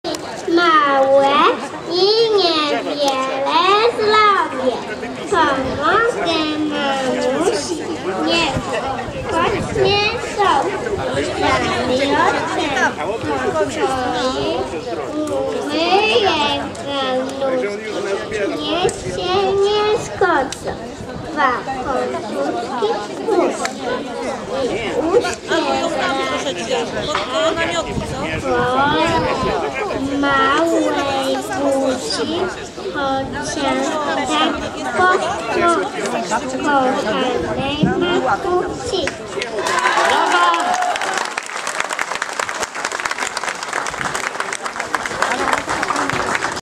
XXI Lubuski Przegląd Zespołów i Grup Śpiewaczych "Pejzaż muzyką malowany", Gościeszowice, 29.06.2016 r., plac przy Gminnym Centrum Kultury